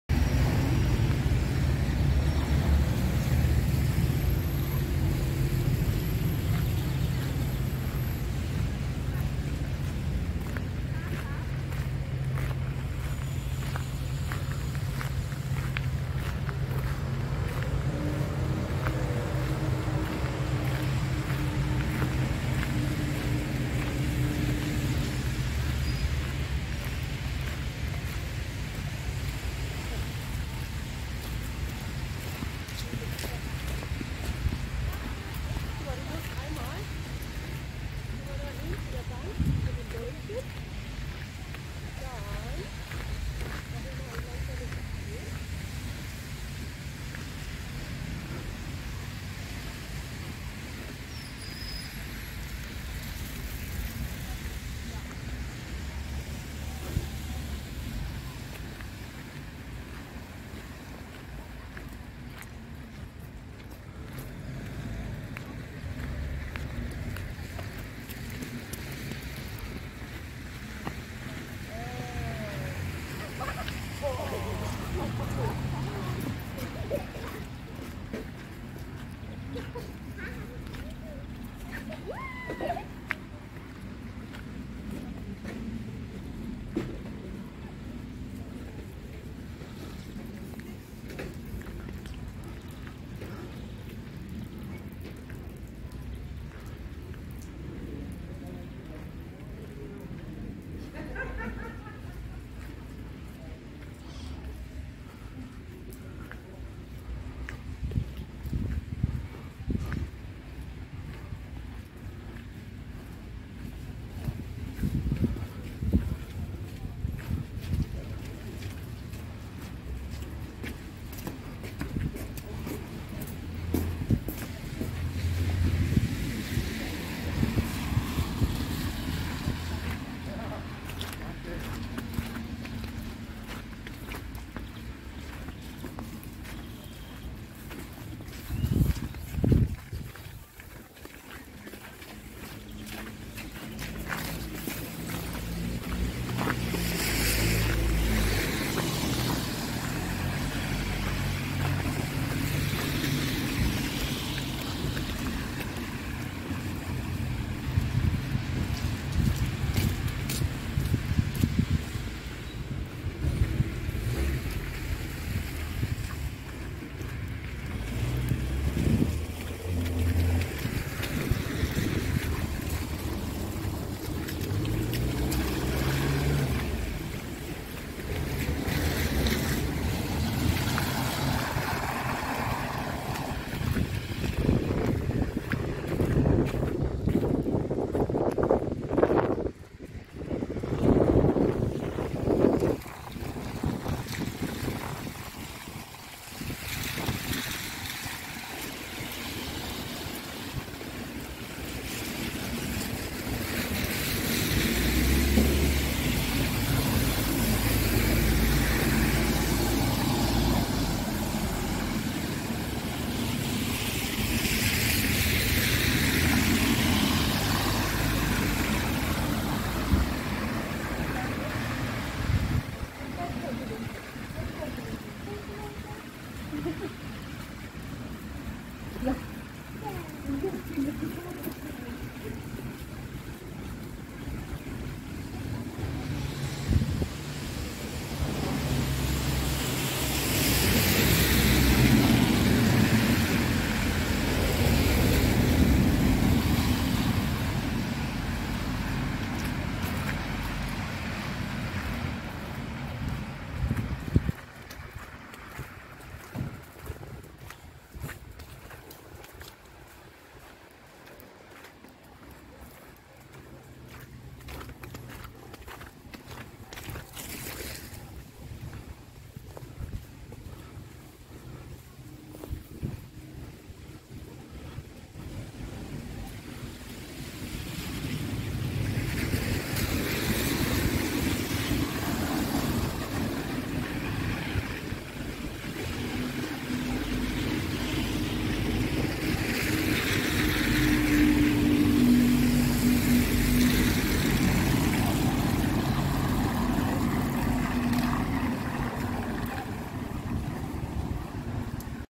Walking in Hamburg _ Germany (Wedel) 31 March 2022
Ein mächtiger Wintereinbruch am Morgen und Vormittag in Wedel: kräftiger Schneefall, in Folge dessen die Temperaturen auf -1°C sanken und sich eine 10-15 cm dicke Schneedecke ausbilden konnte. Das, was der Winter 21/22 nicht hinbekommen hat, wurde heute ziemlich imposant nachgeholt. Mit Abzug des Schneefallgebietes stiegen die Temperaturen rasch an, alles andere wäre für Ende März aber auch unrealistisch gewesen. Auch am Abend zeigt sich die Schneedecke noch vielerorts geschlossen.